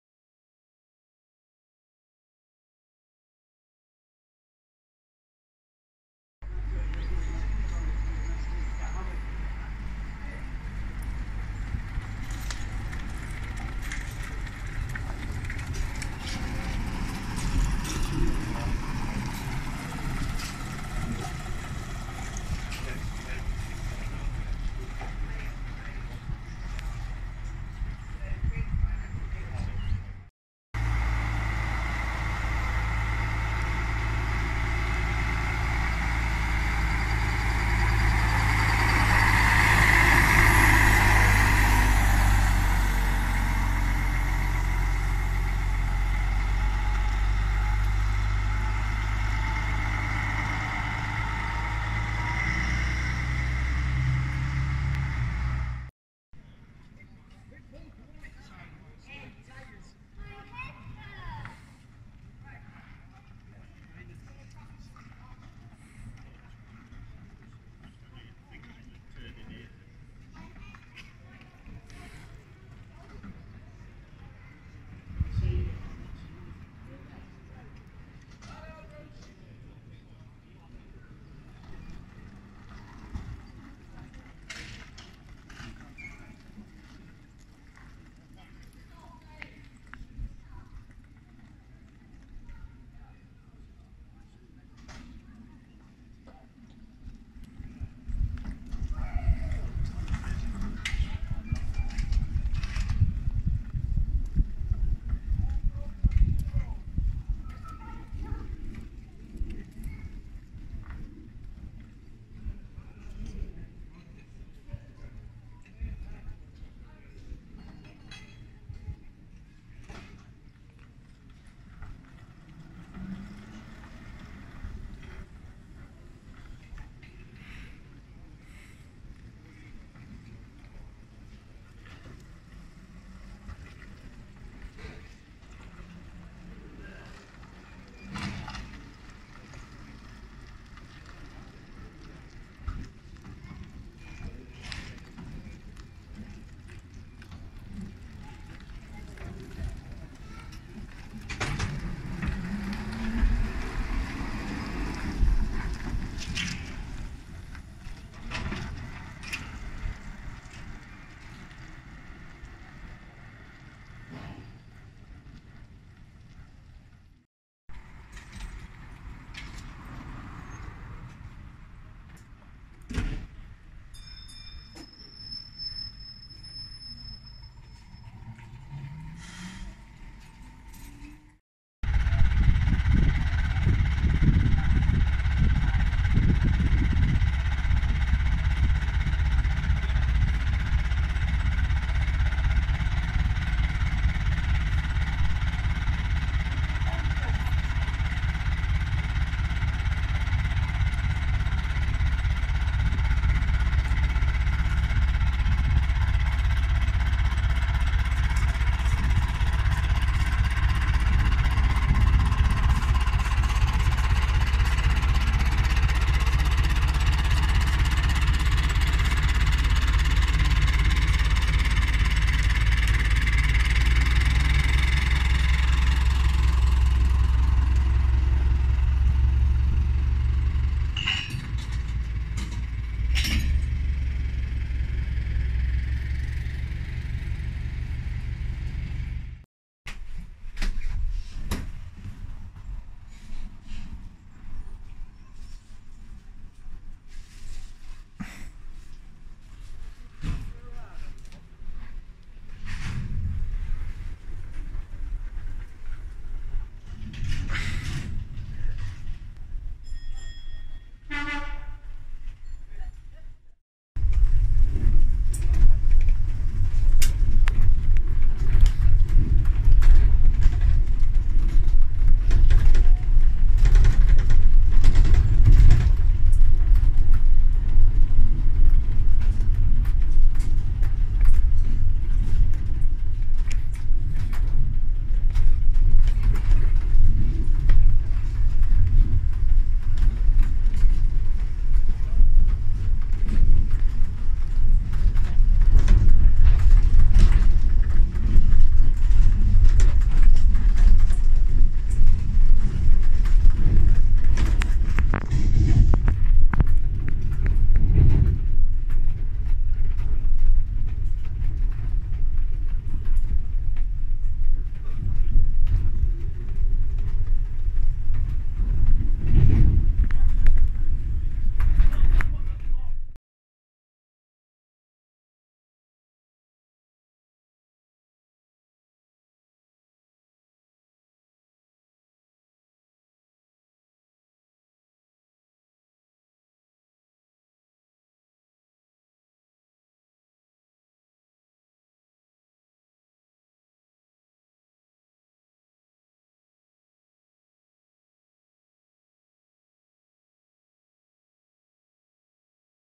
Sandtoft Trolleybus Museum 50th Anniversary Gala
A little video from The Trolleybus Museum at Sandtoft's 50th Anniversary event weekend, filmed on 26 August 2019.

Some video of a Class 142 Pacer ride on my way to the event is